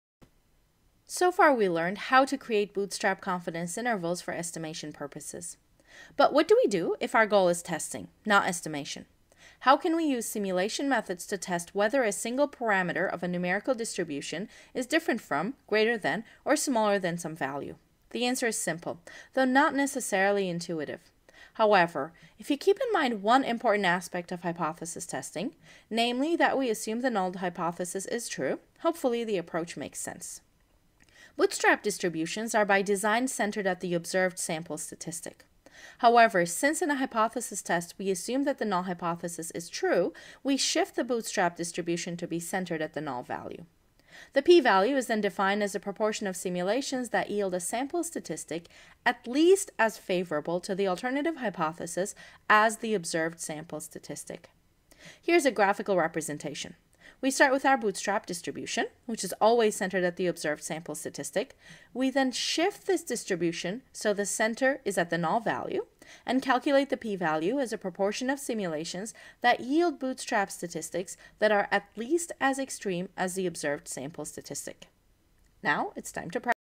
R Tutorial: Re-centering a bootstrap distribution for hypothesis testing
Want to learn more? at your own pace. More than a video, you'll learn hands-on coding & quickly apply skills to your daily work.

So far we learned how to create bootstrap confidence intervals for estimation purposes. But what do we do if our goal is testing, not estimation? How can we use simulation methods to test whether a single parameter of a numerical distribution is different from, greater than, or smaller than some value? The answer is simple, though not necessarily intuitive. However, if you keep in mind one important aspect of hypothesis testing -- namely that we assume the null hypothesis is true -- hopefully the approach makes sense.

Bootstrap distributions are by design-centered at the observed sample statistic.

However since in a hypothesis test we assume that the null hypothesis is true, we shift the bootstrap distribution to be centered at the null value.

The p-value is then defined as the proportion of simulations that yield a sample statistic at least as favorable to the alternative hypothesis as the observed sample statistic.

Here is a graphical representation: We start with our bootstrap distribution, which is always centered at the observed sample statistic. We then shift this distribution so the center is at the null value, and calculate the p-value as the proportion of simulations that yield bootstrap statistics that are at least as extreme as the observed sample statistic.

Now it's time to practice.